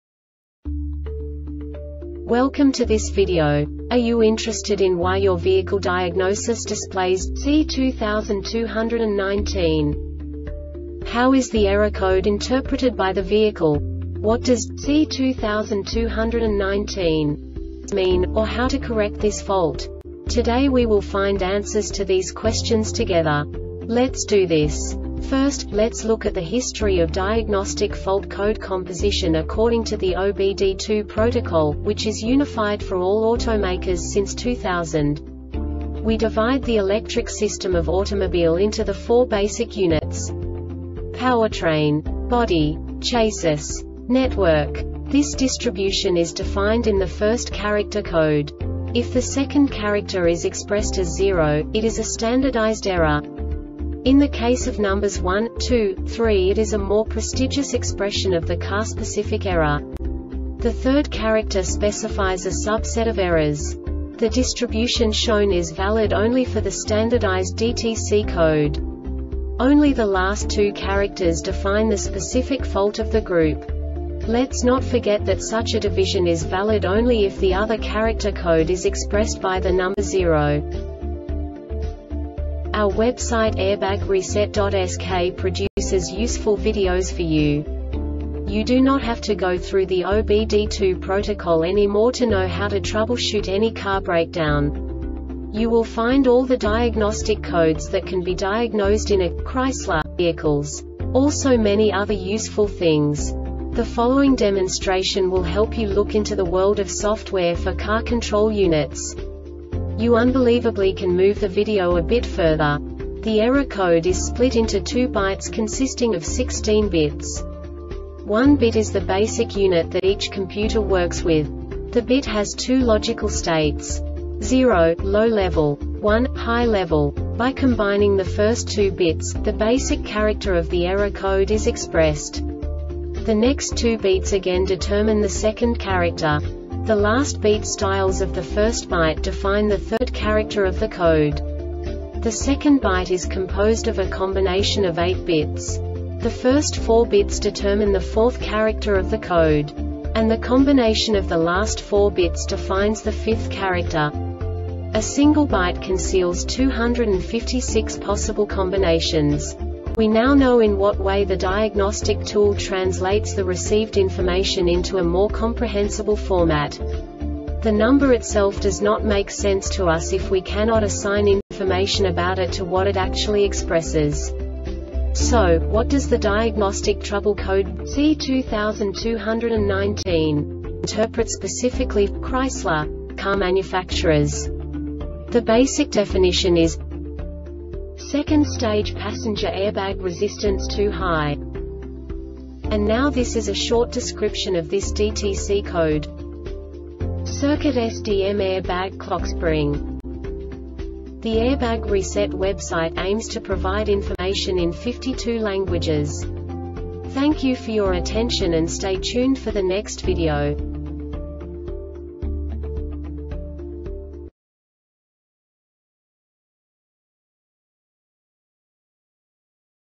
DTC cadillac C2219 Short Explanation
Contents:
0:21 Basic DTC analysis according to OBD2 protocol standard.
1:48 Insight into programming
2:58 A diagnostically understandable description of the Diagnostic Trouble Code
3:05 Basic error definition

Make:
Chrysler
Code:
C2219
Definition:
2nd Stage Passenger Airbag Resistance Too High
Cause:
CircuitSDMAir bagClockspring